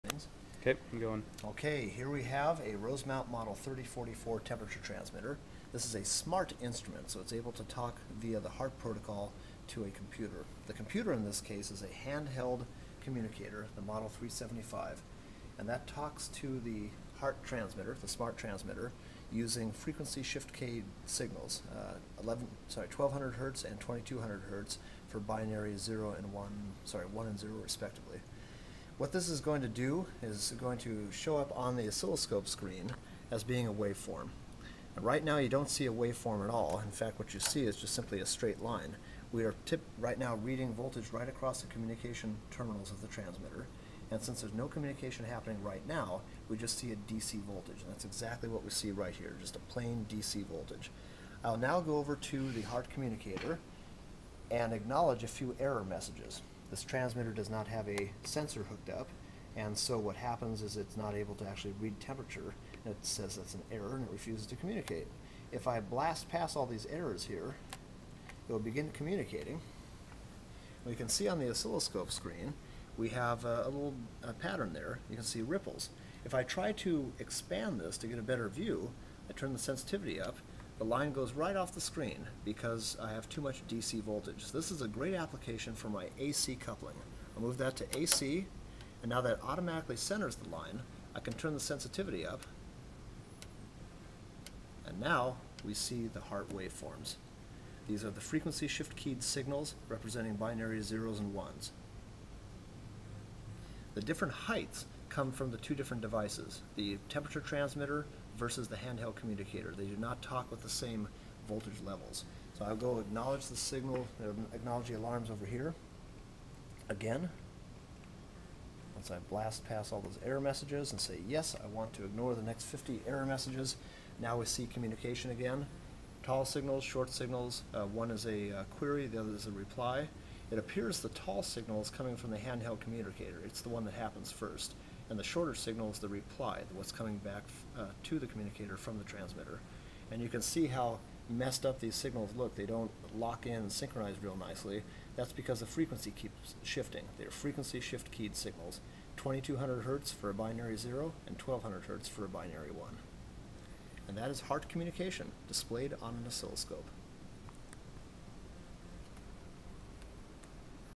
HART signals on oscilloscope
A demonstration of HART communication signals (FSK 2.2 kHz and 1.2 kHz) to and from a "smart" temperature transmitter displayed on an oscilloscope.